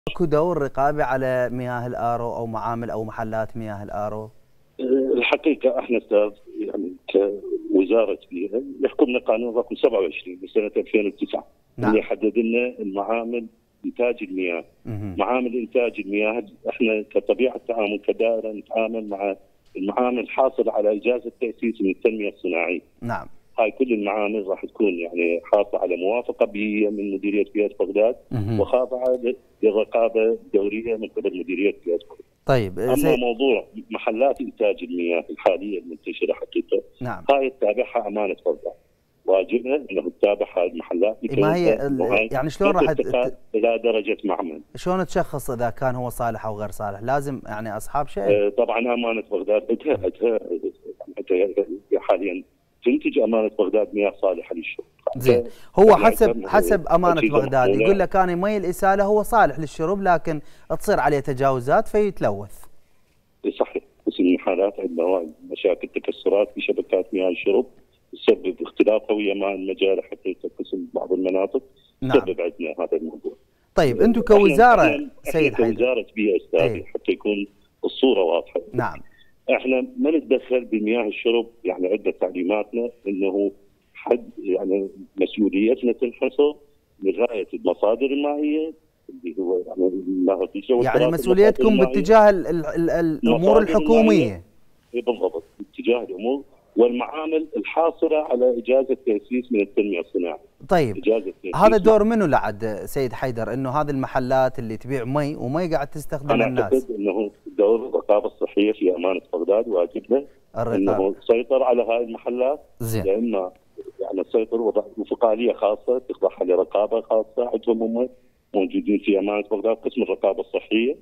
المهندس حيدر يوسف : قسم الرقابة الصحية في دائرة امانة بغداد هو المسؤول على متابعة محلات تصفية المياه
المهندس حيدر يوسف / معاون مدير بيئة بغداد

AP©
AFP©
SNTV©